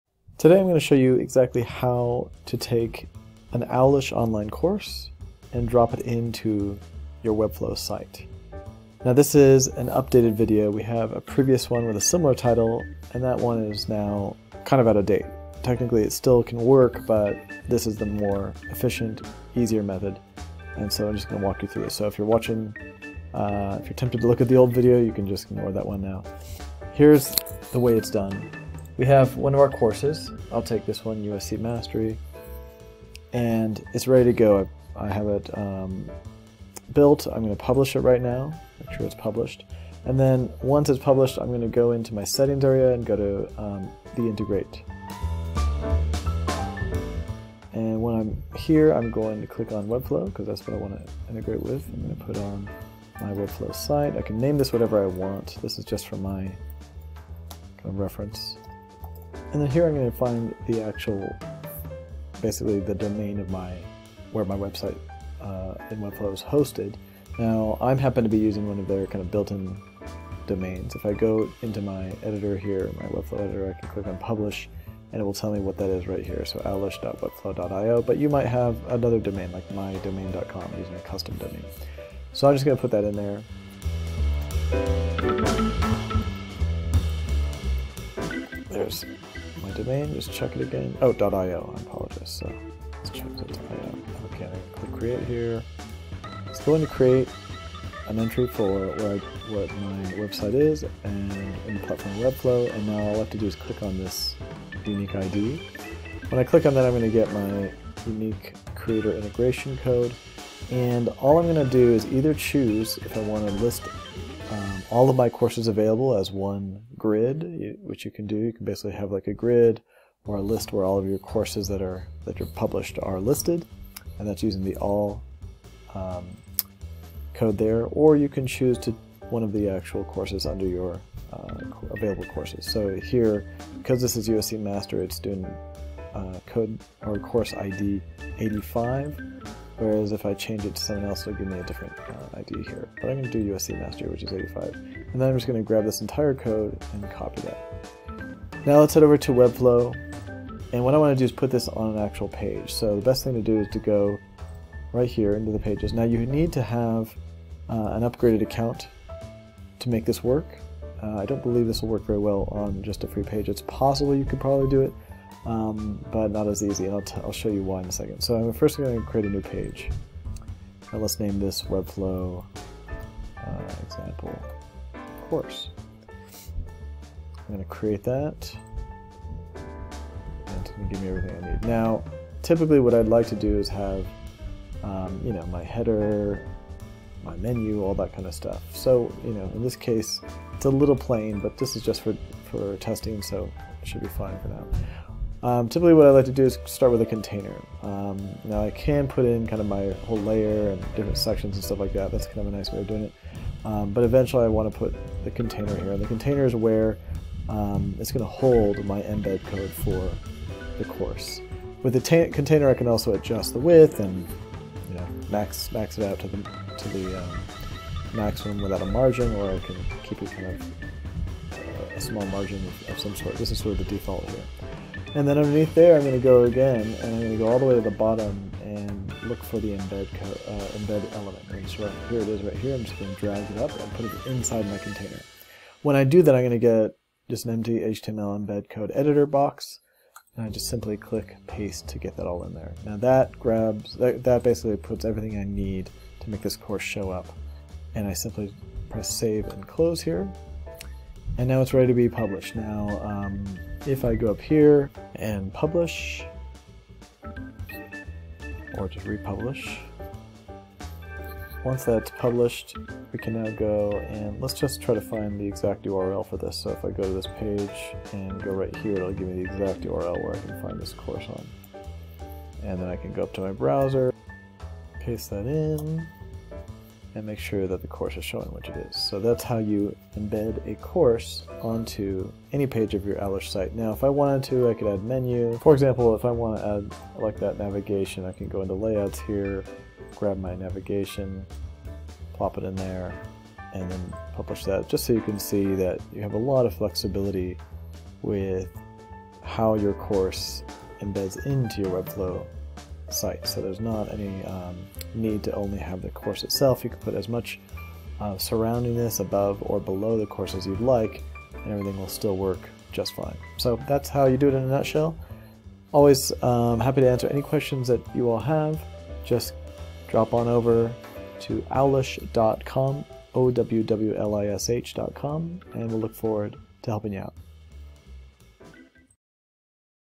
How to Create an Online Course for your Webflow site
Instead of just storing course content files on Webflow store, turn a section of your Webflow site into a functional classroom by creating Owwlish courses and hosting them in Webflow with these quick and easy steps!

No need for time-consuming coding, just integrate and host!